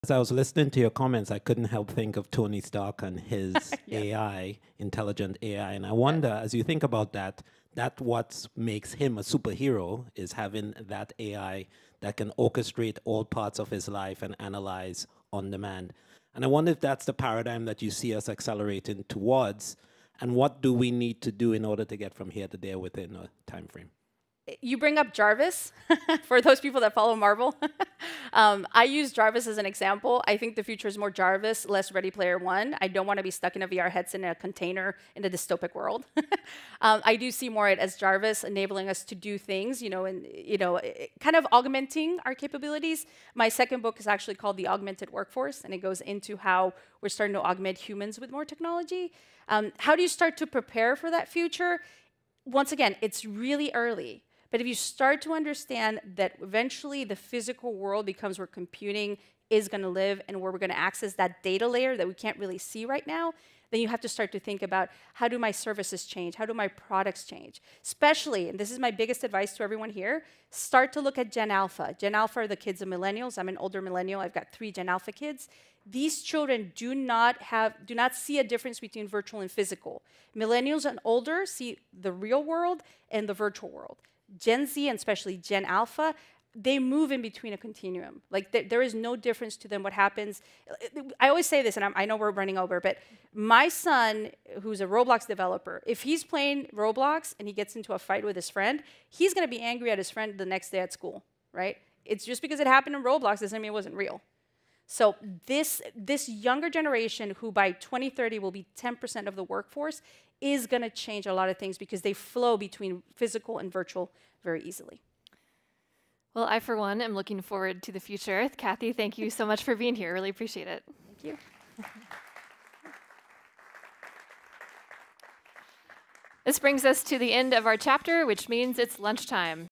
The Future of AI and XR is More Like Iron Man's JARVIS and Less Ready Player One
Explore how AI, like Tony Stark's Jarvis, is reshaping our world. Discover the seamless integration of technology in daily life and what it means for Generation Alpha as they blur the lines between virtual and physical realities.